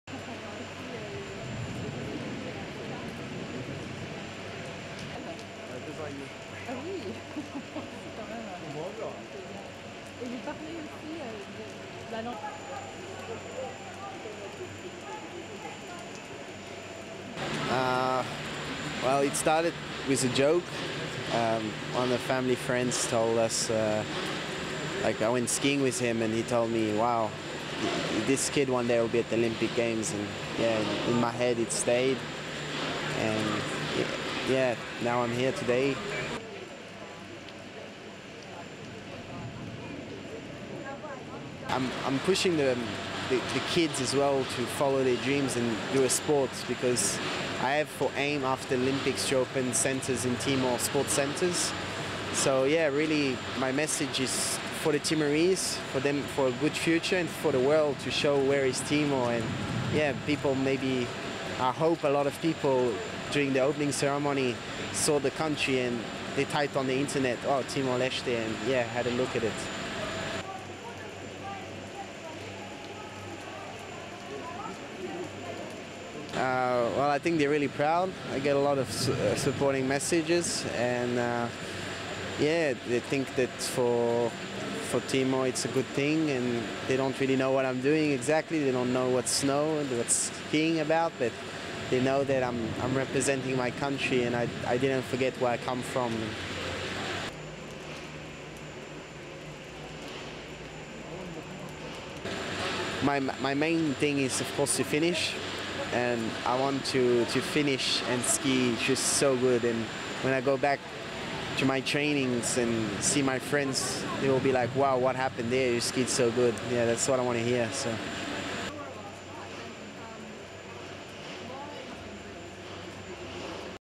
Alpine skier Goutt reflects on being his country's sole athlete at Games
(18 Feb 2014) East Timor's first winter Olympian has been preparing to compete as an Alpine Skier in the Sochi Games.
Nineteen-year-old Yohan Goncalves Goutt, who founded East Timor's officially recognised ski federation, will compete on Saturday night.
"Well, it started with a joke," said Goncalves Goutt.
On a family ski trip in his native France, when he was eight, a family friend said if he kept at the sport, one day he could make it to the Olympics.
Goncalves Goutt said those words stayed in his head: "And now I am here today."
He sees his role in Russia as twofold, he's an athlete, but he's also a sort of ambassador for East Timor, the impoverished half-island nation that was a Portuguese colony, was invaded by Indonesia in 1975, and became a sovereign state in 2002 after the United Nations intervened.
"My message is for the Timorese, for the future and for the world to show where is Timor", he said.
"I hope a lot of people during the opening ceremony saw the country, and they typed on the internet oh, Timor-Leste (East Timor), and had a look at it."
"I am pushing the kids as well to follow their dreams and do a sport, because my aim after the Olympics is to open Centres in Timor, sports centres," Goncalves Goutt said in an interview with The Associated Press on Tuesday.
Born in Paris to a French father, who is in the import-export business, and a Timorese mother, who works full-time to help her skiing son, Goncalves Goutt carries both passports.
He is often asked why he didn't try to compete for France.
But Goncalves Goutt knows how difficult it would have been to make that talented team, as opposed to being a team of one.
And he says he gets positive feedback from Timorese.
"They (Timorese) don't really know what I am doing exactly, they don't know what snow or what skiing is about, but they know that I am representing my country and I did not forget where I come from," Goutt said.
Goncalves Goutt qualified for the Sochi Olympics based on his skiing results.
While he's never competed in a top-level World Cup race, he did finish 14th out of 43 entrants in a slalom in Iran last month.
"My main thing is of course to finish, and I want to finish and ski just so good, and when I go back to my trainings and see my friends, they will be like 'wow, what happened there, you skied so good', yes, that's what I want to hear," he said with a smile.